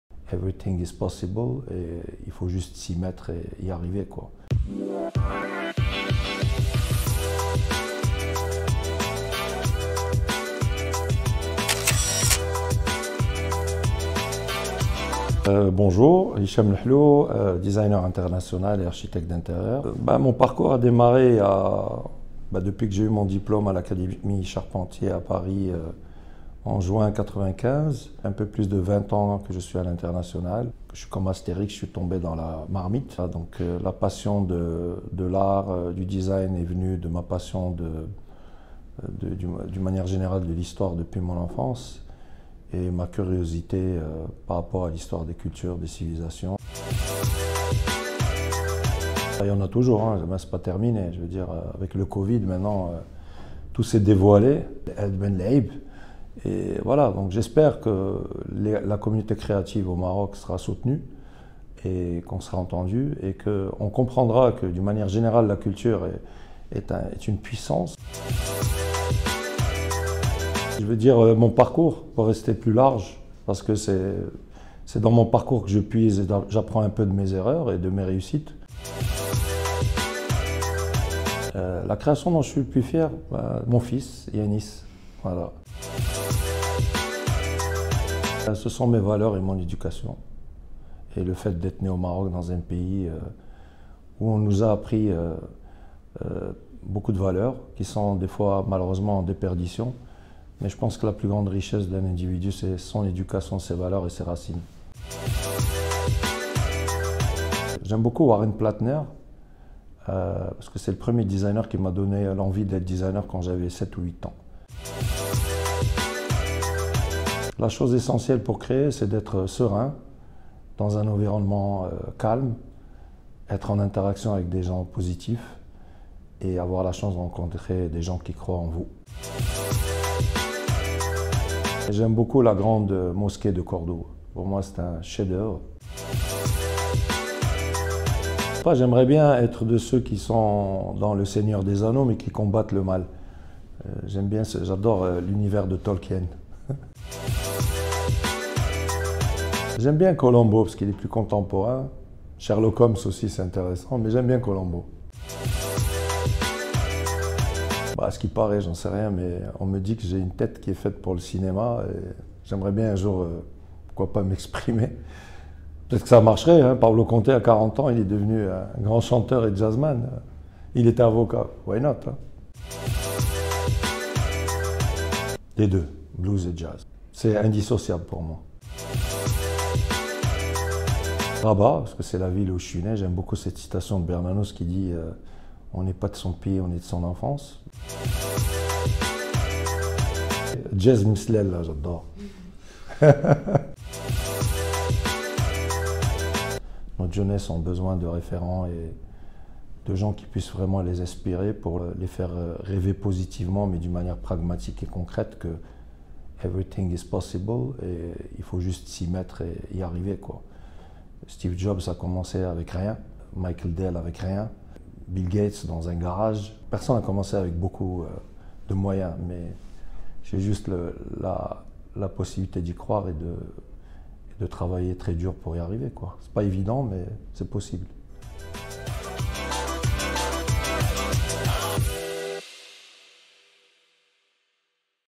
L'interview de Hicham Lahlou, Designer International Marocain
Hicham Lahlou, Designer international marocain, connu pour avoir popularisé le design et véhiculé l’image d’un Maroc contemporain et créatif.
Hicham nous raconte son parcours, ses réussites ainsi que ses obstacles.